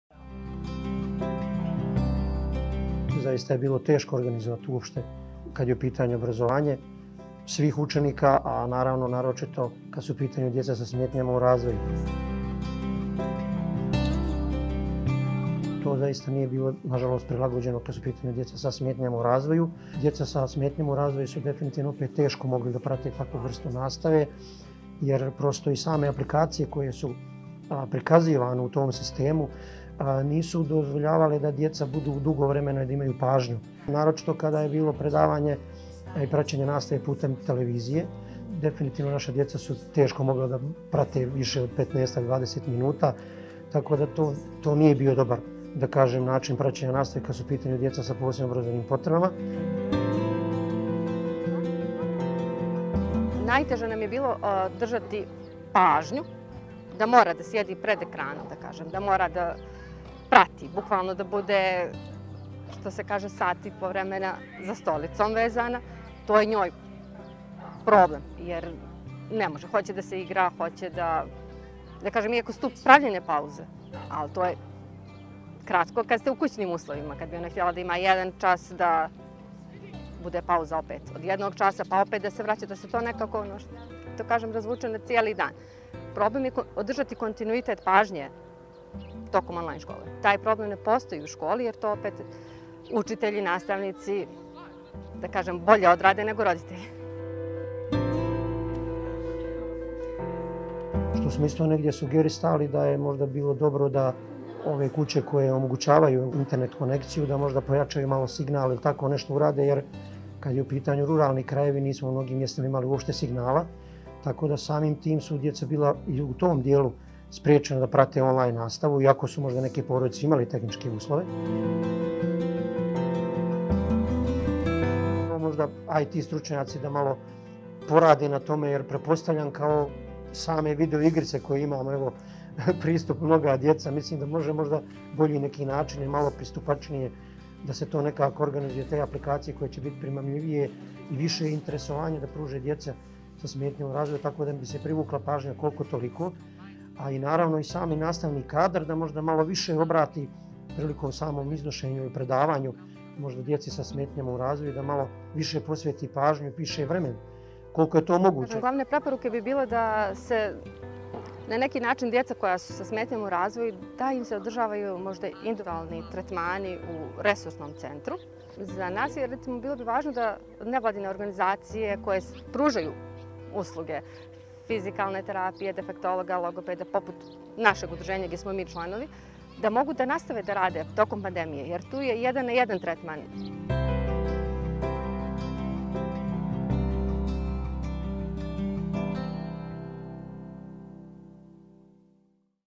Roditelji djece sa smetnja u razvoju o obrazovanju tokom pandemije I UNICEF Montenegro
👉 Za više informacija o radu UNICEF-a u Crnoj Gori, klik na